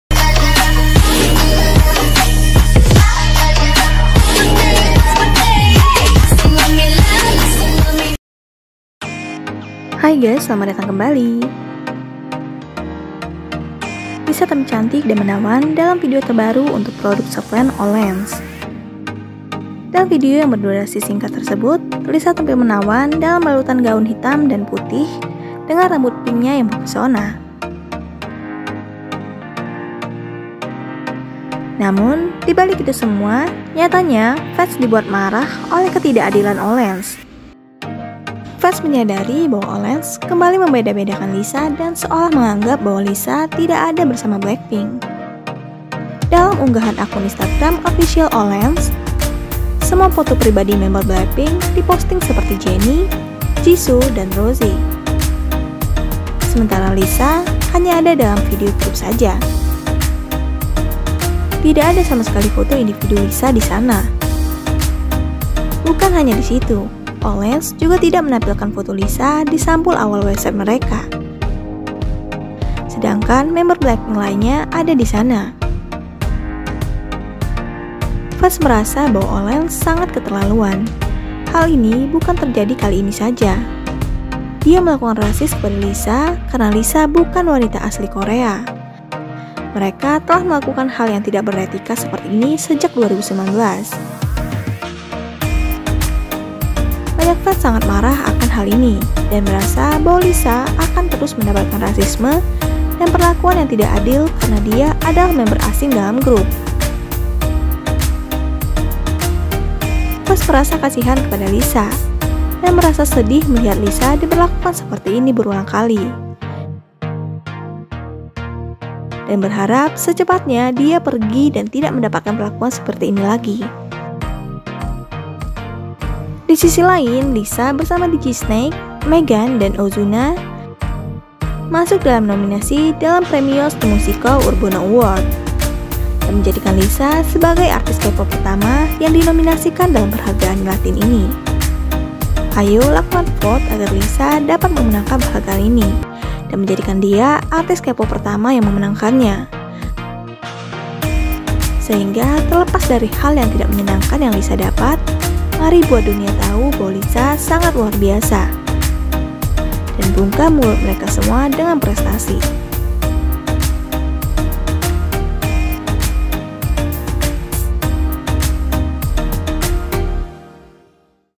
LISA KEMBALI MENDAPATKAN PERLAKUAN YANG TIDAK ADIL, FANS SANGAT MARAH 🤬
Fans lisa kembali dibuat marah akan perlakuan yang tidak adil dari brand olens, simak selengkapnya di video.

cr : YG entertainment, olens